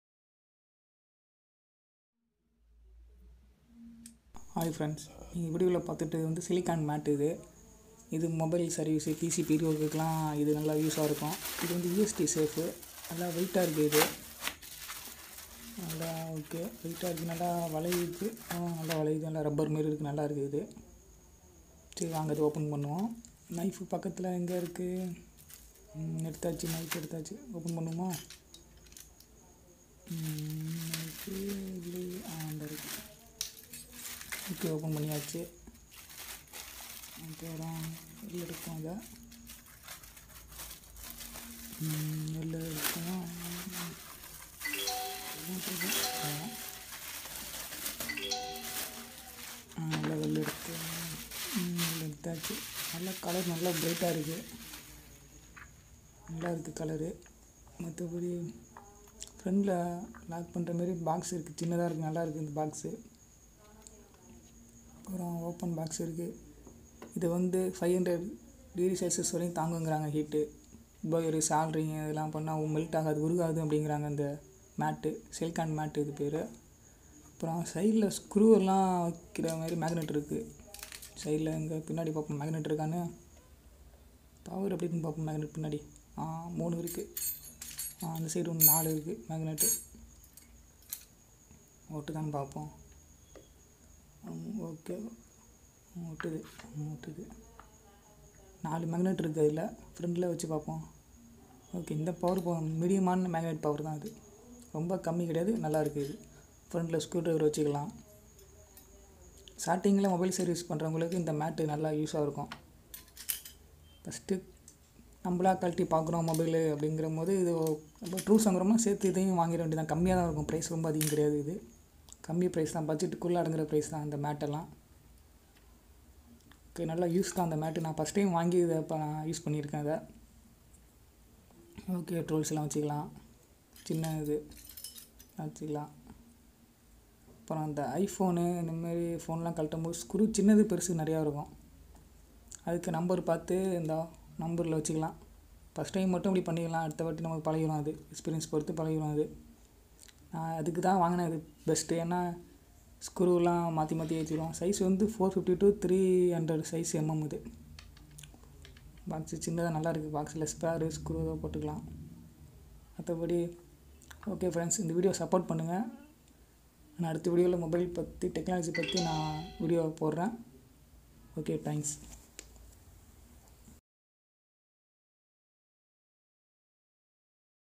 silicone mat soldering and repiair mat unboxing review tamil
A must have accessory for your workbench. Premium Quality Magnetic ESD Safe Mat. Large Size - 450 mm x 300 mm. Make your repair work easy.
Silica gel Thickness: approx. 5 mm. Heat Insulation Pad. Resistant to high temperature of up to 500 °C.
It has special areas for small screws, IC Chip and other small parts.
Also, Suitable for working with hot air gun and for Heat Gun BGA Soldering stations
Built-in scale ruler (0 to 36 cm ) on top section